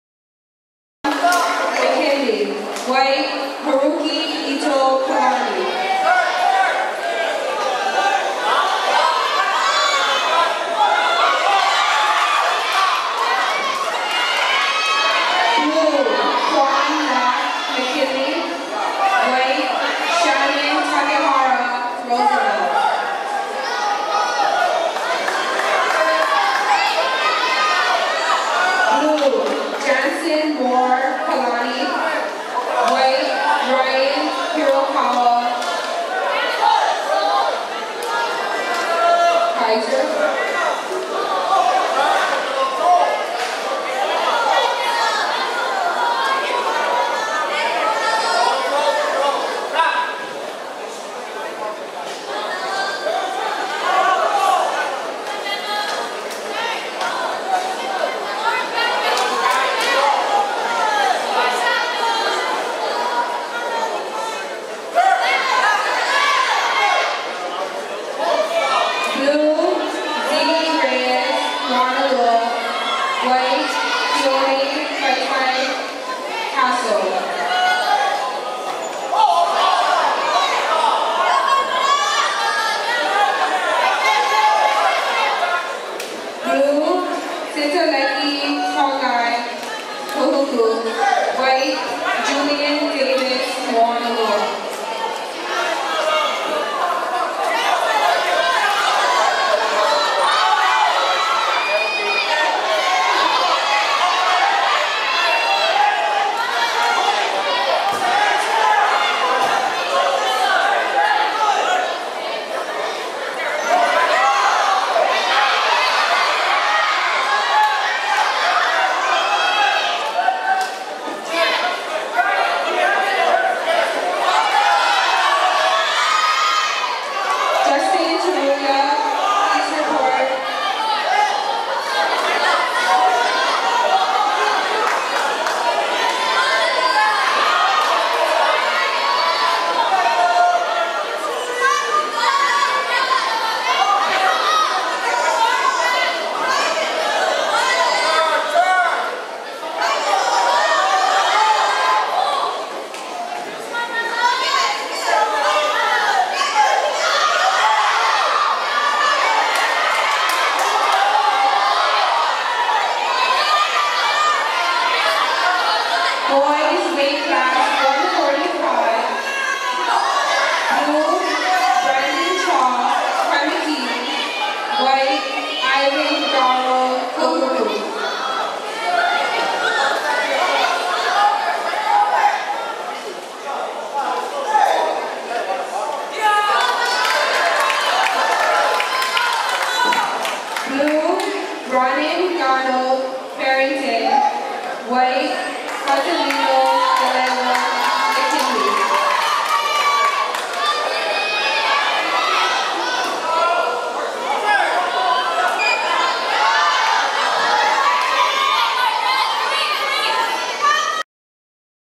J3 Girls - OIA East Judo Tournament 4-18-15
click2ed videos **

2015 OIA East Division Judo Tournament
April 18, 2015 - McKinley High School Gym

Castle Knights, Farrington Governors, Kahuku Red Raiders, Kaimuki Bulldogs, Kaiser Cougars, Kalaheo Mustangs, Kalani Falcons, McKinley Tigers, Moanalua Na Menehune, Roosevelt Rough Riders